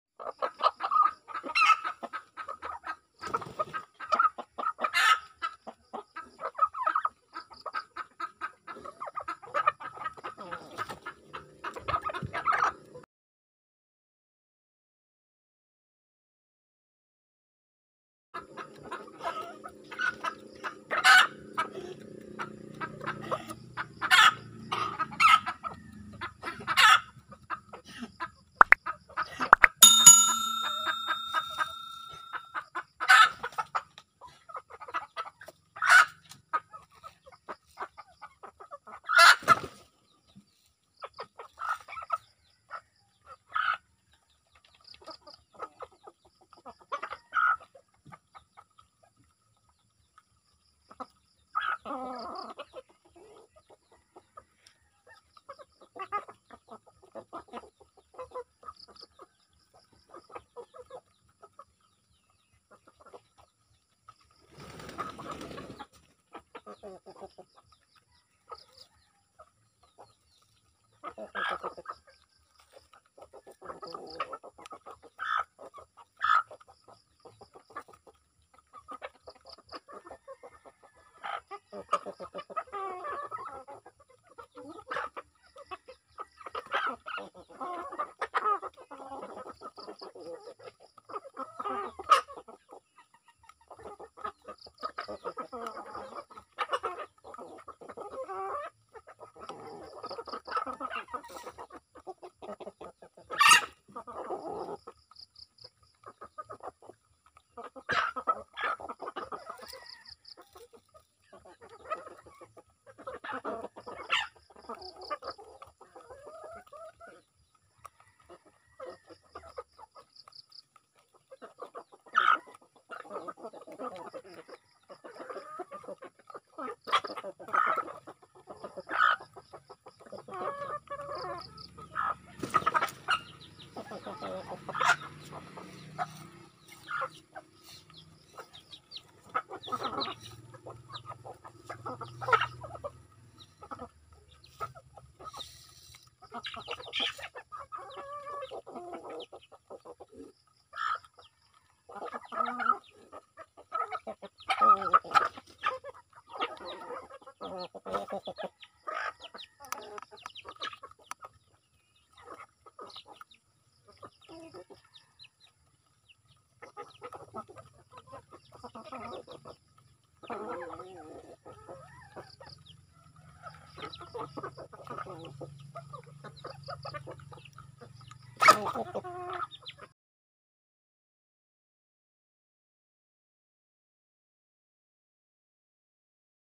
Ganito ako mag-alaga ng manok|Lily Gutz Mix Vlog
Madali lang mag-alaga ng mga manok dito sa probinsiya.